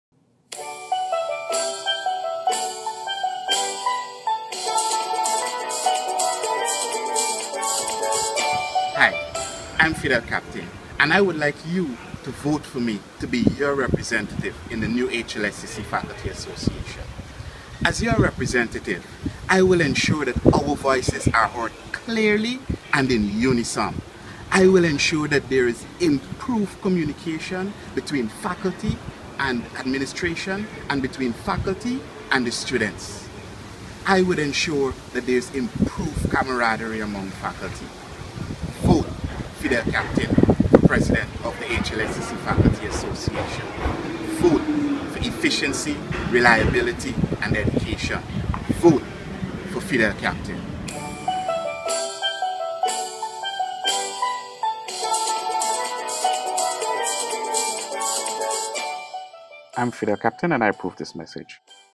Vote Fidel
Vote Fidel Captain for President of the HLSCC Faculty Association. Vote for efficiency, reliability, and dedication.